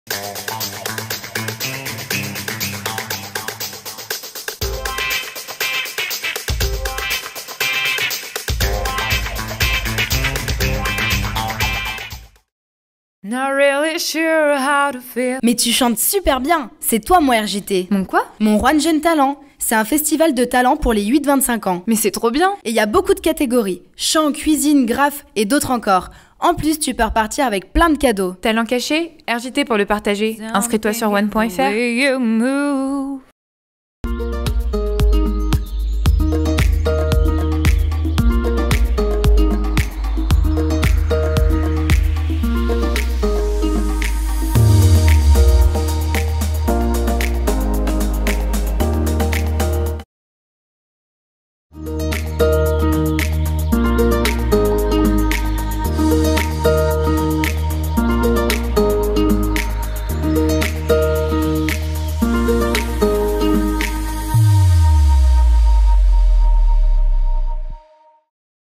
Roanne Jeunes Talents - Spots d'Or 2019
Créations publicitaires réalisées dans le cadre de la formation pour le DUT Techniques de commercialisation à l'IUT de Roanne. Ces vidéo peuvent être utilisées commercialement par le commanditaire mais l’objectif pédagogique est d’initier les étudiants aux stratégies de la communication commerciale, à partir d’une réalisation concrète. En plus des compétences techniques en audiovisuel et création graphique, les étudiants mettent en pratique leurs connaissances acquises en cours de négociation, marketing, expression, droit, anglais, communication commerciale,…